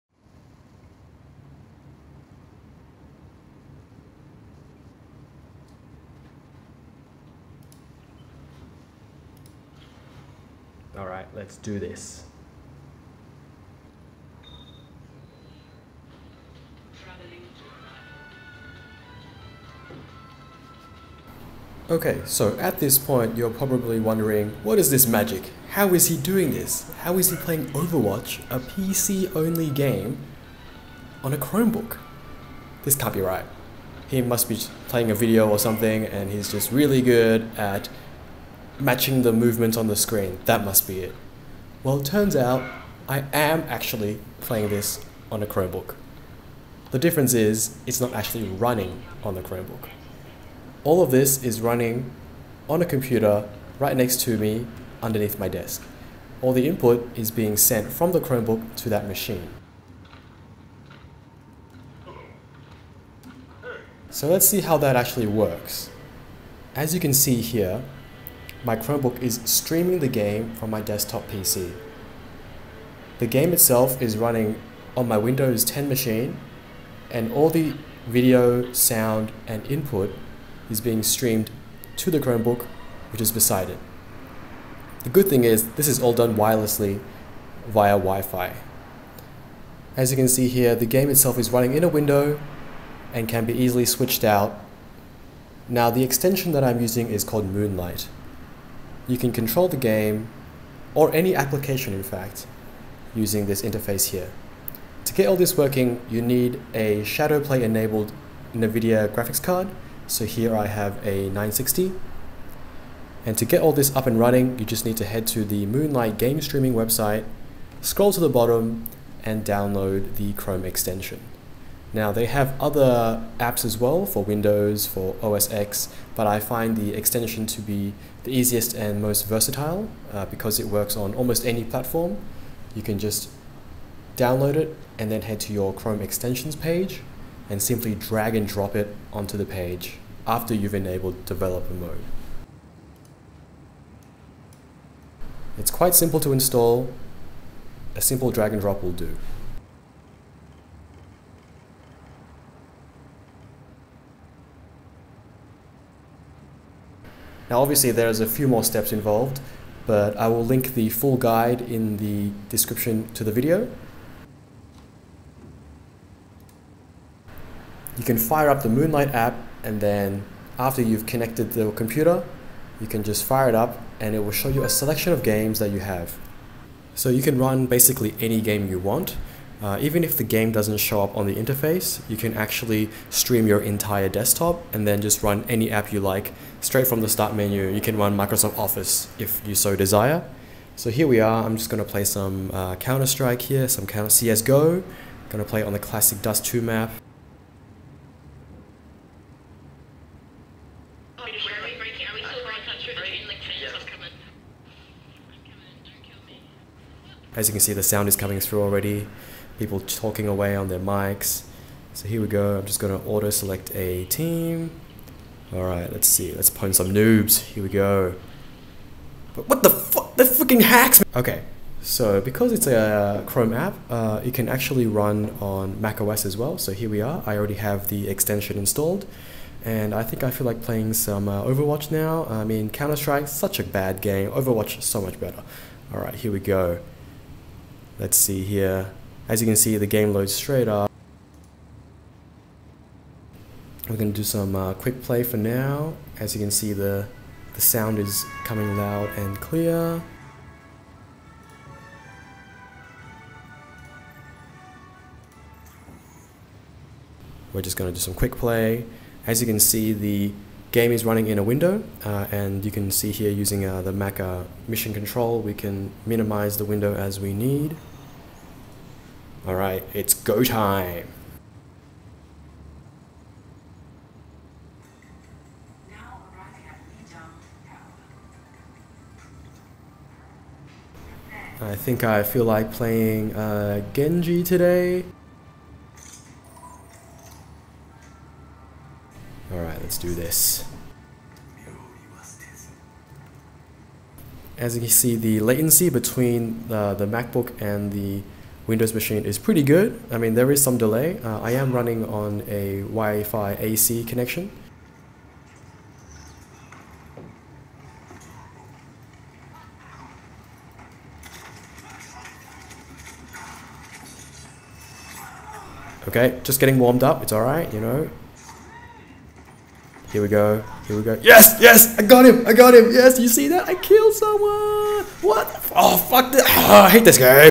How to play PC games through Chrome!
Have you ever wanted to play PC only games on a Mac, or a Chromebook, or heck on your iOS and Android device? Look no further, this is your solution